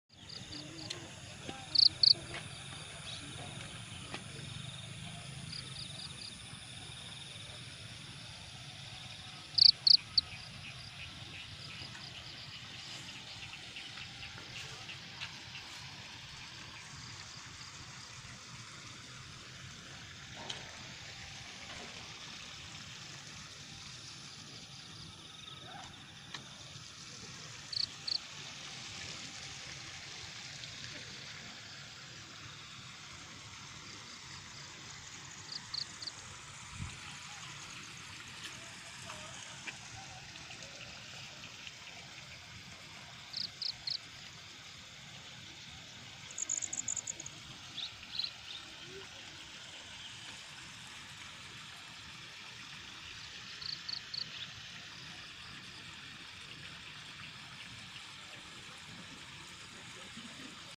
Penampakan di embung nglanggeran gunungkidul Yogyakarta
Embung nglanggeran

Adalah embung yg dibuat dekat dengan wisata gunung api purba nglanggeran